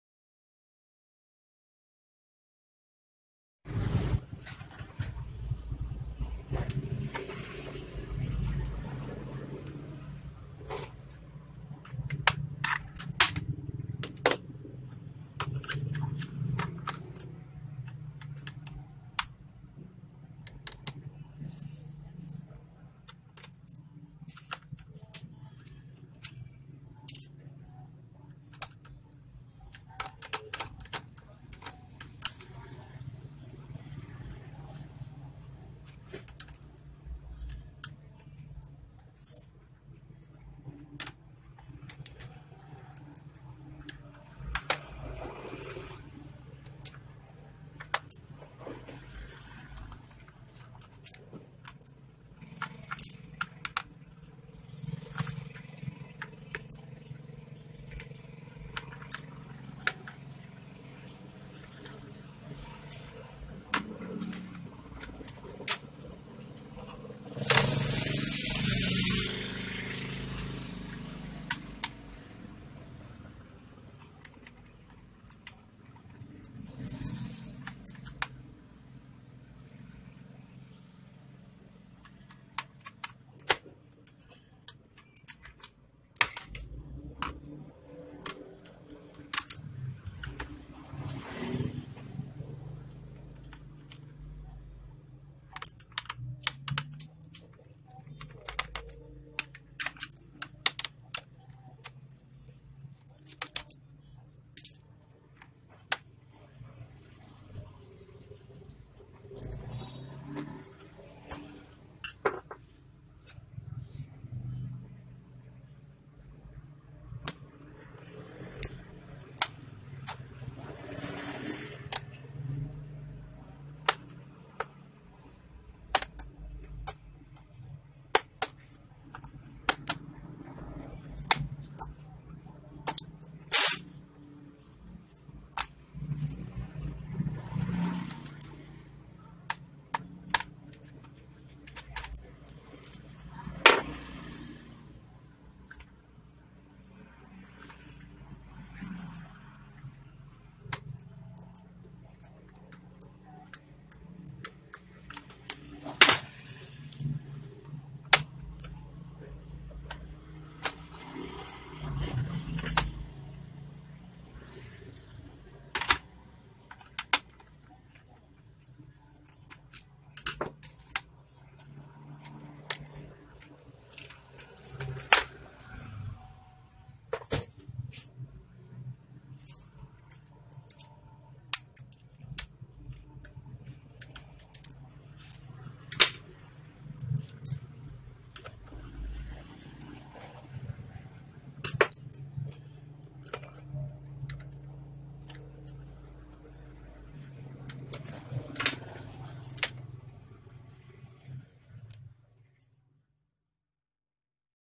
How to disassemble Acer Aspire 4752 laptop
This video will show you how to disassemble Acer Aspire 4752 laptop. if you want to repair this video will help you to remove it.